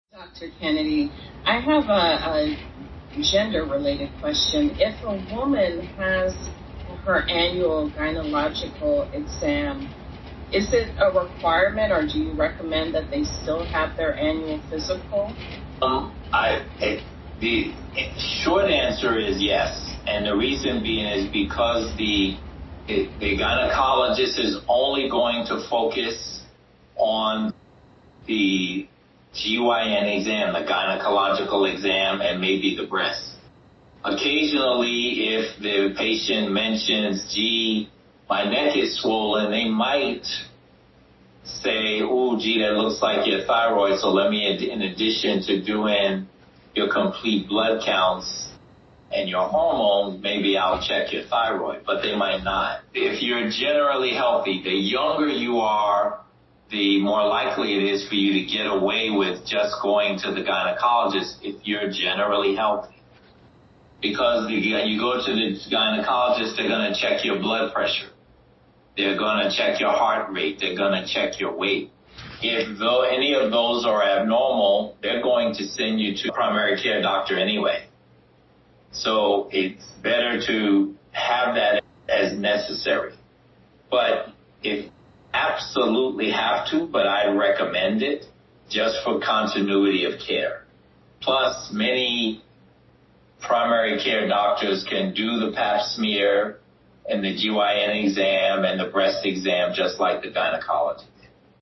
If a woman gets an annual gynecological exam, should she still get a physical?
• Contact our co-host "Aussie" Mike James and sign up for our newsletter: Wellness Musketeers Newsletter